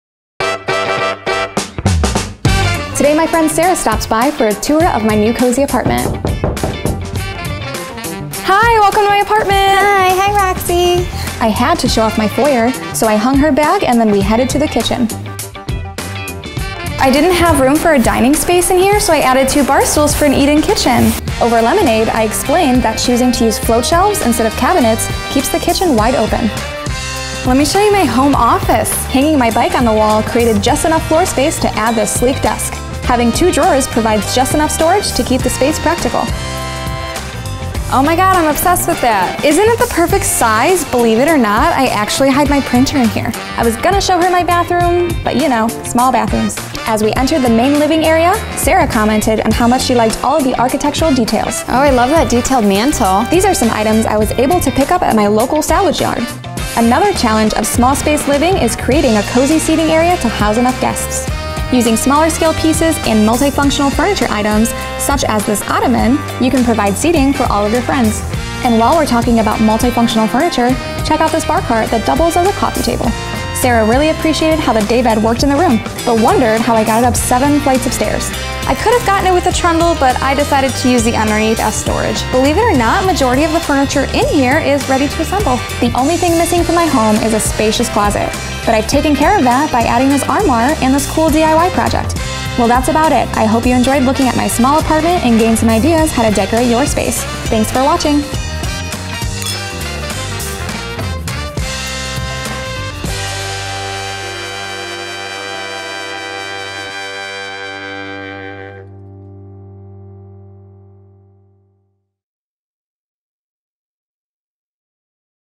Inspire Q Small Apartment Living
Want more style inspiration?
When space is tight, it may seem difficult to be able to furnish your space to accommodate all your wants and needs. In this video, iNSPIRE Q demonstrates how small space living can be not only easy, but chic and styled all at once!
Products Featured
• Foyer